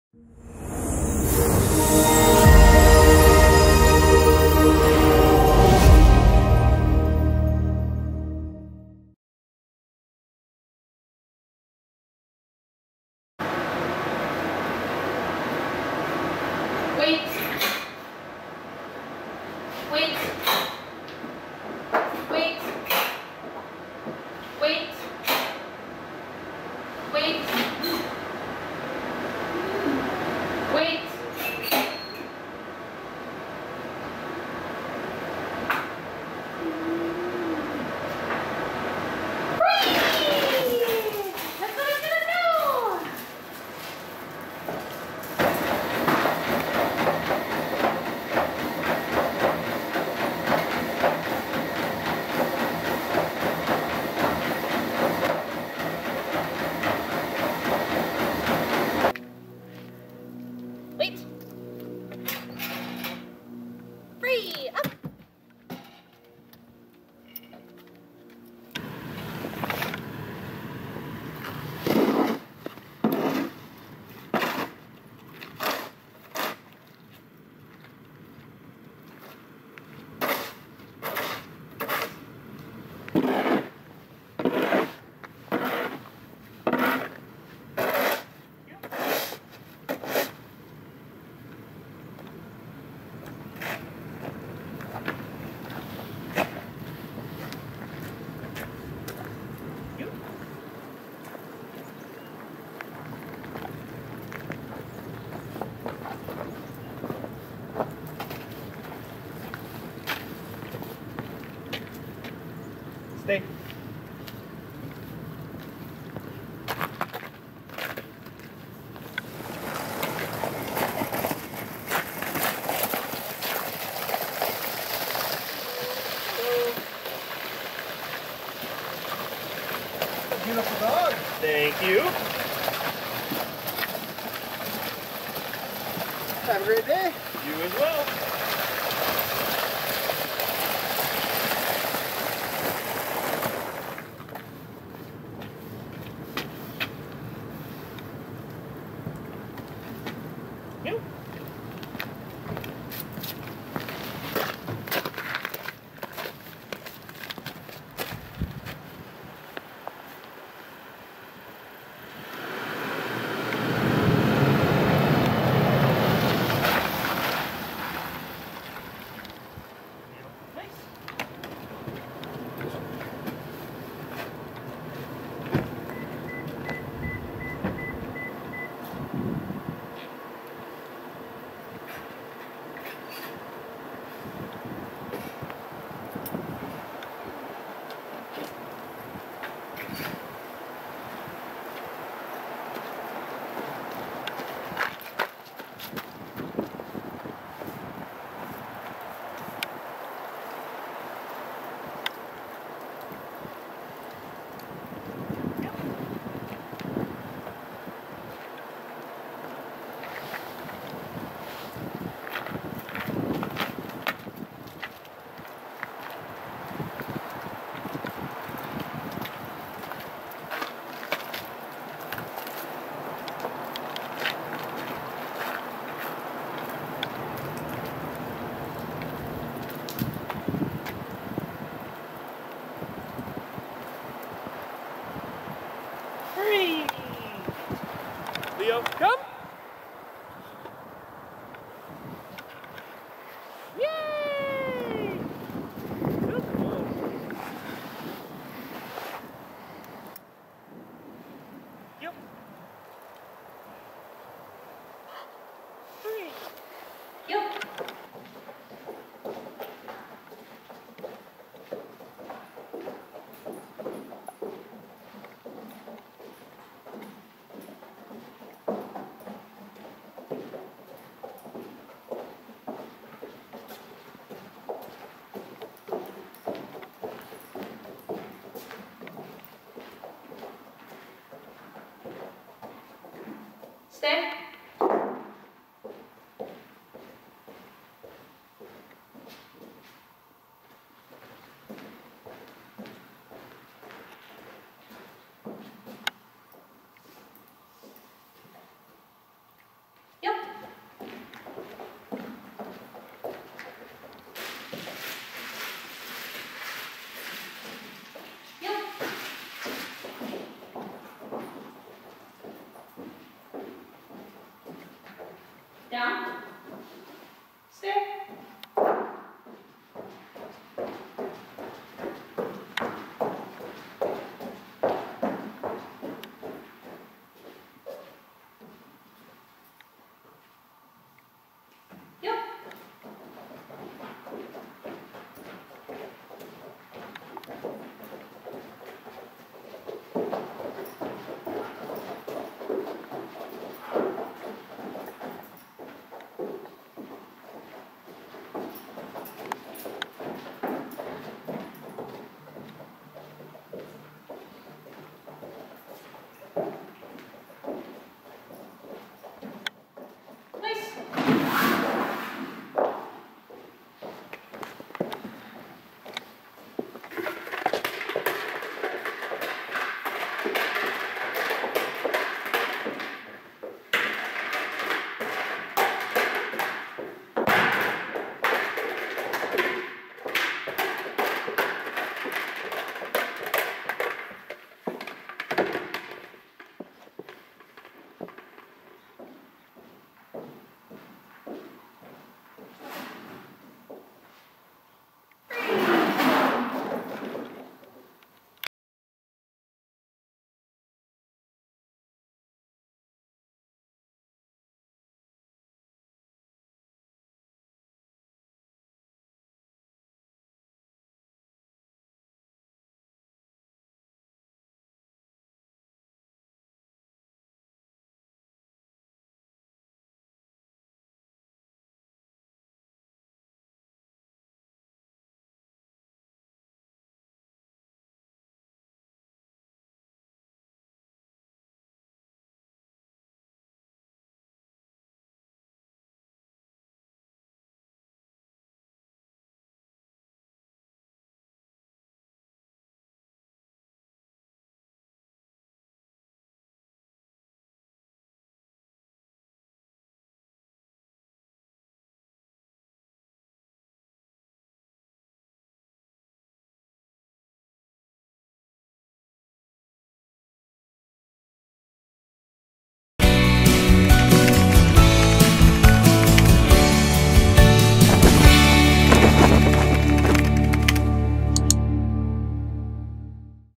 Leo (Golden Retriever) Puppy Camp Dog Training Video Demonstratin
Leo (Golden Retriever) graduated from the Neuman K-9 Academy. During this course dogs learn name-recognition, sit, down, stay, place command, drop-it, walking on-leash without pulling, not jumping on the handler, getting in or out of a vehicle on command, waiting at doors, going into a kennel on command, and socialization with other friendly dogs.